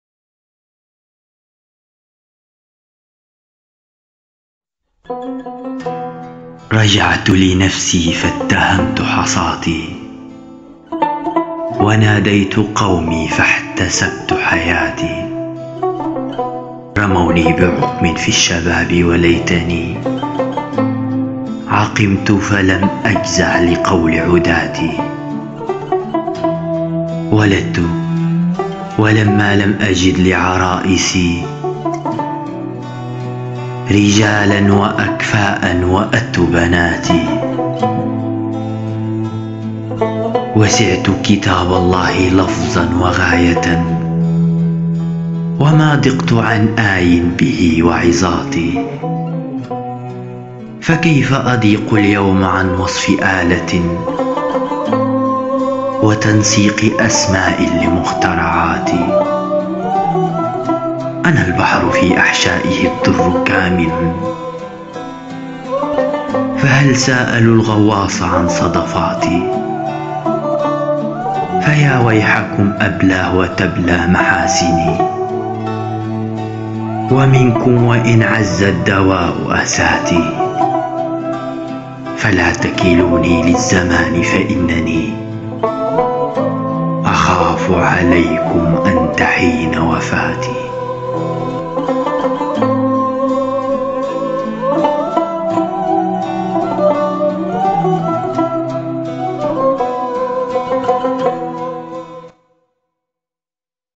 حافظ إبراهيم - أَنا البَحرُ في أَحشائِهِ الدُرُّ كامِنٌ- بصوت الوسمي عبدالله
أَنا_البَحرُ_في_أَحشائِهِ_الدُرُّ_كامِنٌ حافظ_ابراهيم القصيدة عمودية ، من بحر الطويل مطلعها
رَجَعتُ_لِنَفسي_فَاِتَّهَمتُ_حَصاتي

نبذة عن الشاعر:
حافظ إبراهيم شاعر مصري من الرواد الأعلام ، و أحد قادة مدرسة الإحياء في نهاية القرن العشرين ، ولد في ديروط بأسيوط عام 1871 أو 1872م ، فقد أباه طفلاً ، فكفله خاله ، التحق بالشرطة ، و ظل فيها لفترة و سافر إلى السودان ثم أحيل إلى الإستيداع